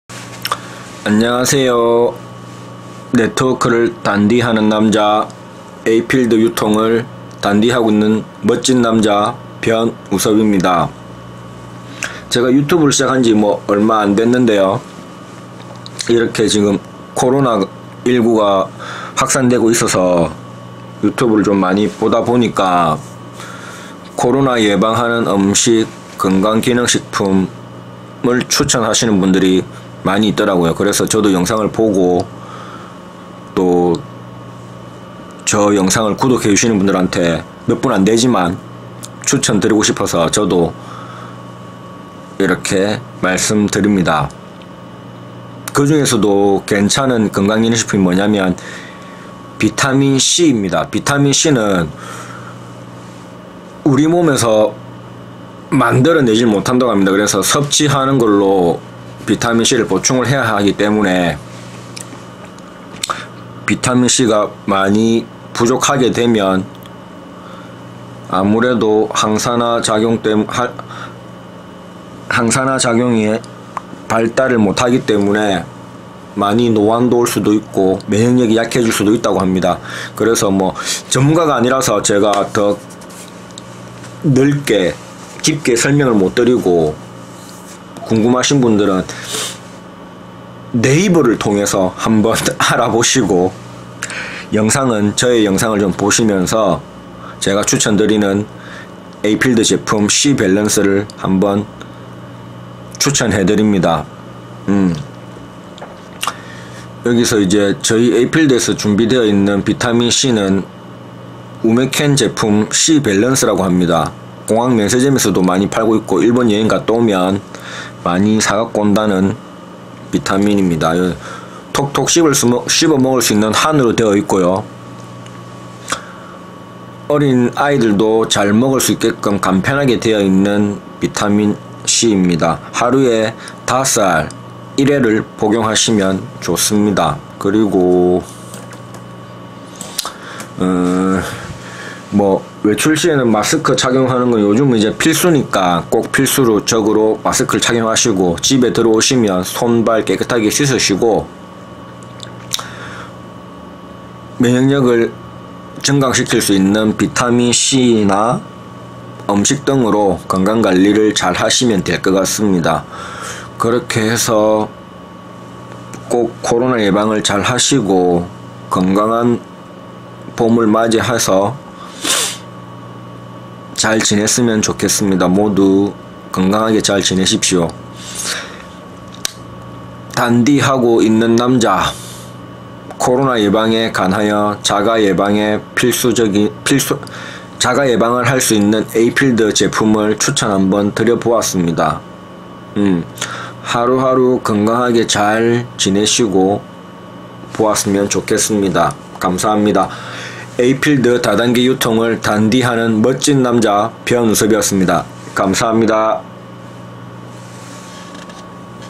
에이필드 비타민c 코로나19 에추천하는 영양제
코로나19 예방을위한 항바이러스 효과를
받을수 있는 비타민C  추천드립니다

에이필드 를 단디 하는 남자 단디 에이필드 dandi 멋쟁이 남자

건강하게 잘이겨내시고 봄을 맞이하세요